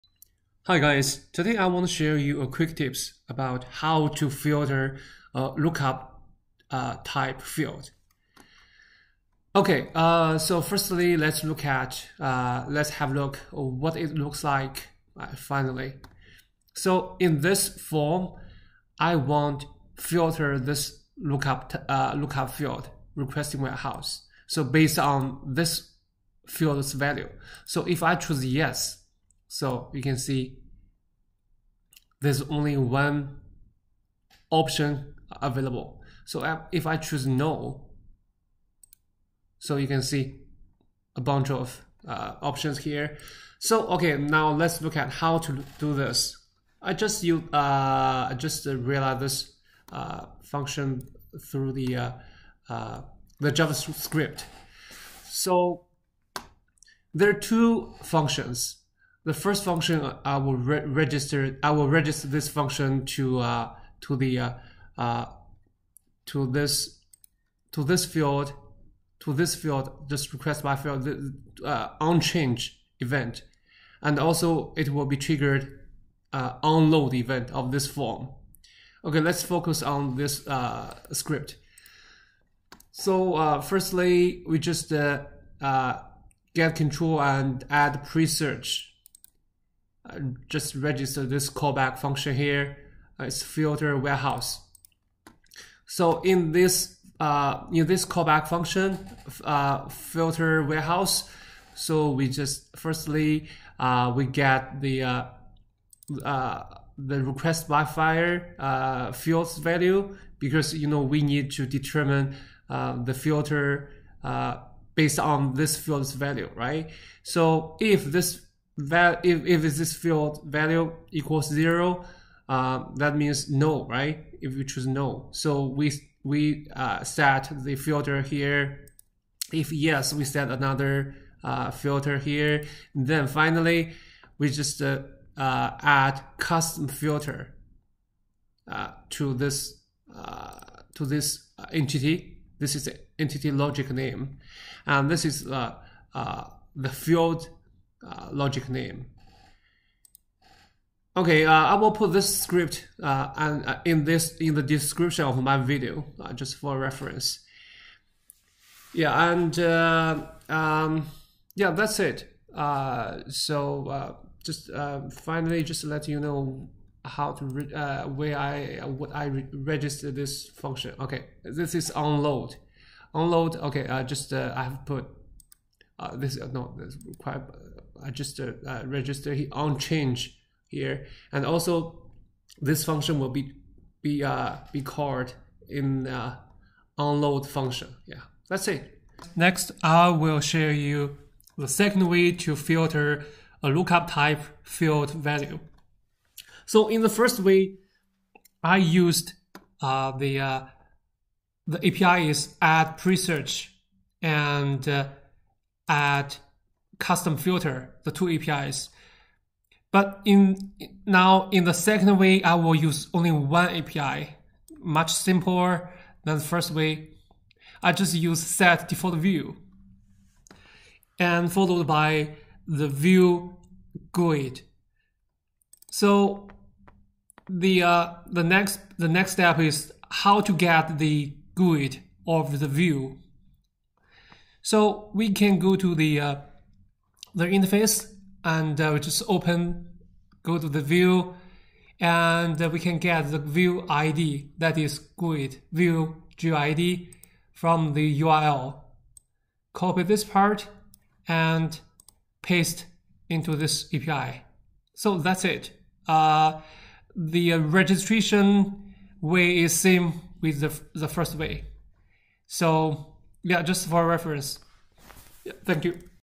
[DEMO] Two ways to Filter Lookup type Field values in Dynamics 365/CE
Solution 1:
Use API:  addPreSearch  &  addCustomFilter
Solution 2:
Use API:  setDefaultView
Details:
There is another example of a complicated scenario which across several entities and filtering from multiple values. Check below url for details.